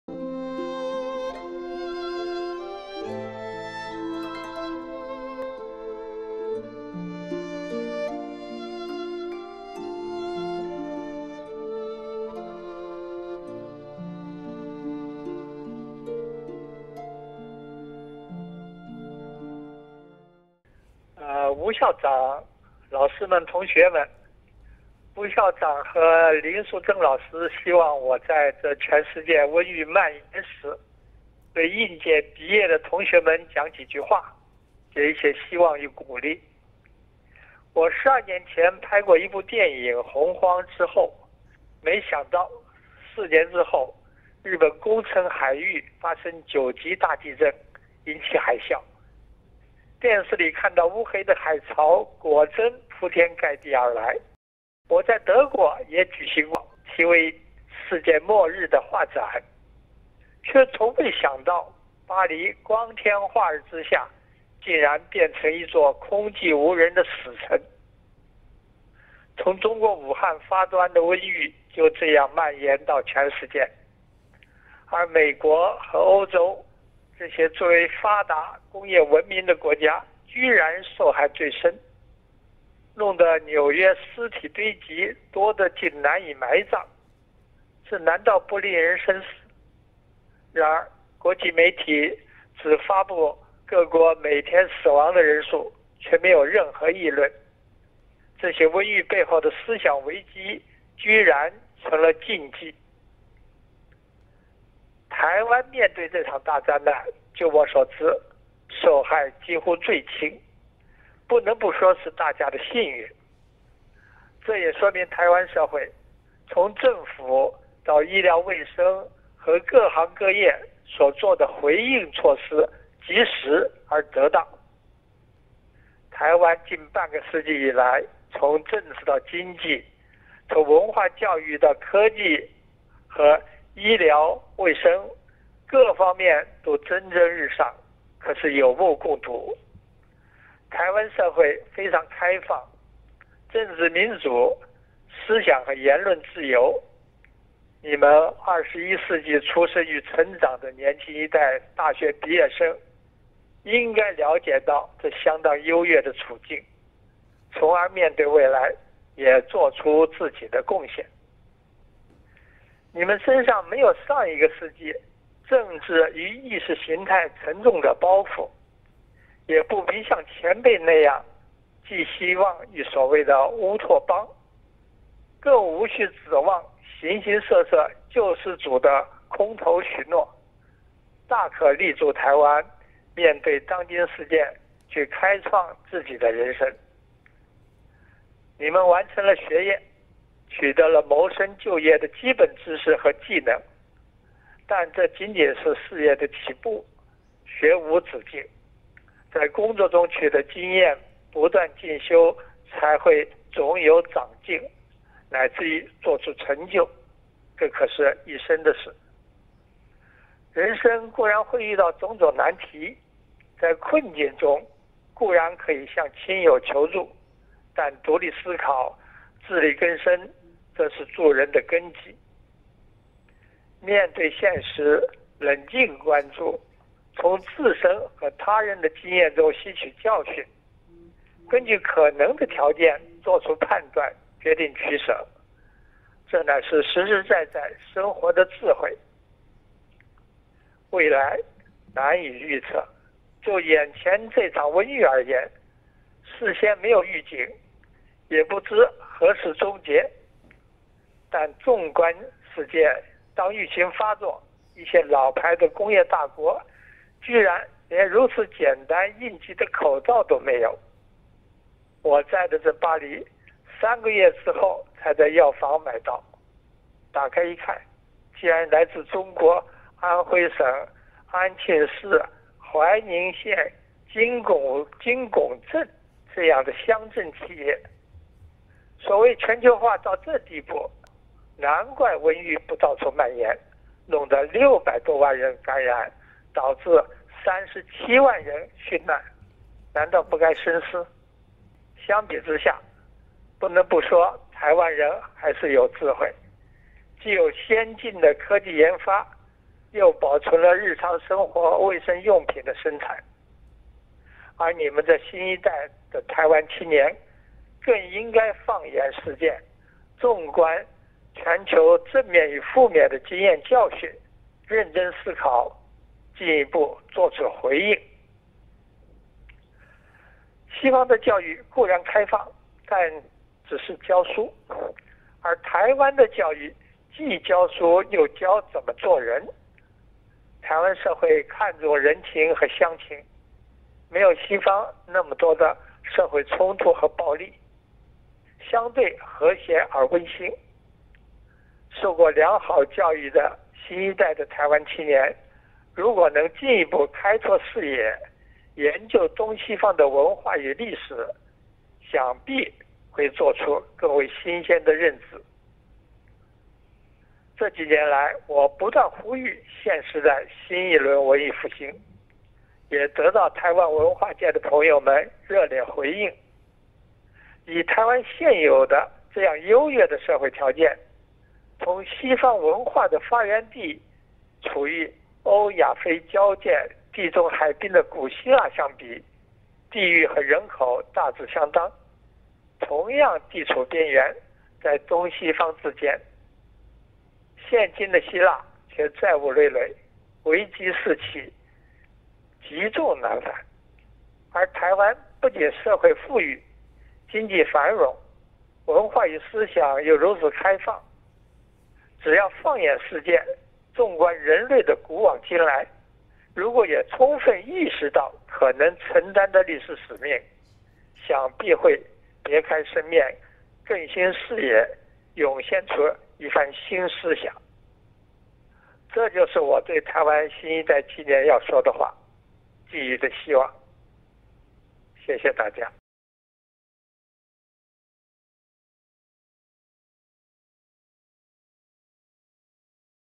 諾貝爾獎得主高行健勉勵師大畢業生：立足臺灣、放眼世界、承擔使命
高行健講座教授於巴黎封城之際跨海給本校109級畢業生的叮嚀全文如下：

吳校長，老師們，同學們：

吳校長和林淑真老師希望我在這全世界瘟疫蔓延時，對應屆畢業的同學們講幾句話，給一些希望與鼓勵。我十二年前拍過一部電影《洪荒之後》，沒想到四年之後，日本宮城海域發生九級大地震，引起海嘯，電視裏看到烏黑的海潮，果真鋪天蓋地而來。我在德國也舉行過題為《世界末日》的畫展，卻從未想到巴黎光天化日之下，竟然變為一座空寂無人的死城。

從中國武漢發端的瘟疫，就這樣蔓延到全世界，而美國和歐州這些最為發達工業文明的國家居然受害最深，弄得紐約屍體堆積，多得竟難以埋葬。這難道不令人深思？然而，國際媒體只發佈各國每天死亡的人數，卻沒有任何議論。這些瘟疫背後的思想危機，居然成了禁忌。

臺灣面對這場大災難，就我所知，受害幾乎最輕，不能不說是大家的幸運。這也說明臺灣社會從政府到醫療衛生和各行各業，所做的回應措施及時而得當。臺灣近半個世紀以來從政治到經濟，從文化教育到科技與醫療衛生，各方面都蒸蒸日上，可是有目共睹。臺灣社會非常開放，政治民主，思想和言論自由。你們二十一世紀出身與成長的年輕一代大學畢業生，應該瞭解這相當優越的處境，從而面對未來，也做出自己的貢獻。

你們身上沒有上一個世紀政治與意識形態沉重的包袱，也不必像前輩那樣寄希望於所謂的烏托邦，更無須指望形形色色救世主的空頭許諾，大可立足臺灣，面對當今世界，去開創自己的人生。

你們完成了學業，取得了謀生就業的基本知識和技能，但這僅僅是事業的起步，學無止境，在工作中取得經驗，不斷進修，才會總有長進，乃至於做出成就，這可是一生的事。

人生固然會遇到種種難題，在困境中固然可以向親友求助，但獨立思考，自立更生，則是做人的根基。面對現實，冷靜關注，從自身和他人的經驗中吸取教訓，根據可能的條件做出判斷，決定取捨，這乃是實實在在生活的智慧。

未來難以預測，就眼前這場瘟疫而言，事先沒有預警，也不知何時終結。但縱觀世界，當疫情發作，一些老牌的工業大國居然連如此簡單應急的口罩都沒有，我在的這巴黎三個月之後才在藥房買到。打開一看，竟然來至中國安徽省安慶市懷甯縣金拱鎮這樣的鄉鎮企業，所謂全球化到這地步，無怪瘟疫不到處蔓延，弄得六百多人感染，導致三十七萬人殉難，難道不該深思？

相比之下，不能不說臺灣人還是有智慧，既有先進的科技研發，又保存了日常生活衛生用品的生產。而你們這新一代的臺灣青年更應該放眼世界，縱觀全球正面與負面的經驗教訓，認真思考，進一步做出回應。

西方的教育固然開放，但是只教書，而臺灣的教育既教書又教怎麼做人。臺灣社會看重人情和鄉情，沒有西方那麼多的社會衝突和暴力，相對和諧而溫馨。受過良好教育的新一代的臺灣青年，如果能進一步開拓視野，研究東西方的文化與歷史，想必會得出更為新鮮的認知。

這幾年來，我不斷呼籲現時代新一輪文藝復興，也得到臺灣文化界的朋友們熱烈回應。以臺灣現有的這樣優越的社會條件，同西方文化的發源地處於歐亞非交界地中海濱的古希臘相比，地域與人口大致相當，同樣地處邊緣，在東西方之間。現今的希臘卻債務累累，危機四起，積重難返。

而臺灣不僅社會富裕，經濟繁榮，文化與思想又如此開放。只要放眼世界，縱觀人類的古往今來，如果也充分意識到可能承擔的歷史使命，想必會別開生面，更新視野，湧現出一番新思想。這就是我對臺灣新一代青年要說的話，寄予的希望。

謝謝大家！

2020年5月30日 巴黎